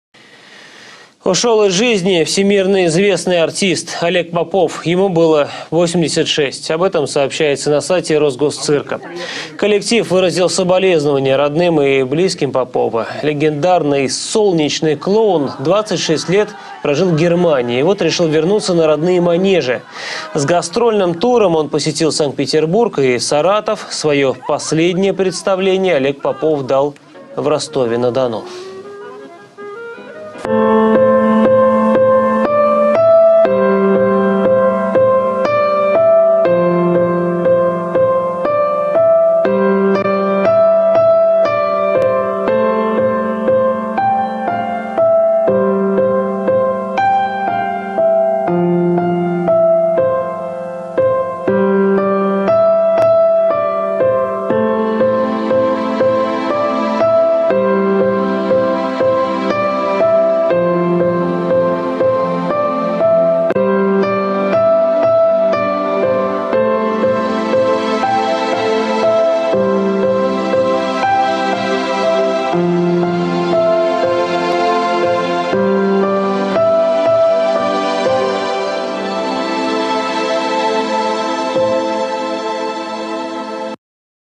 Умер всемирно известный клоун Олег Попов -03.11.2016
На 87-году жизни умер всемирно известный клоун, народный артист СССР Олег Попов. Знаменитый артист скончался поздно вечером в цирковой гостинице в Ростове-на-Дону, где был на гастролях